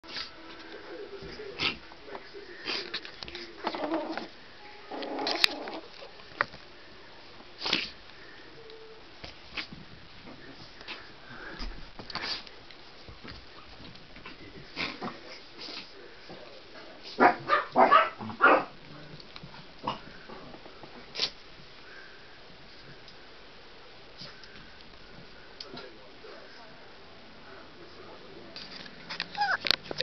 Dori playing April '11
My little 3-legged Maltese playing.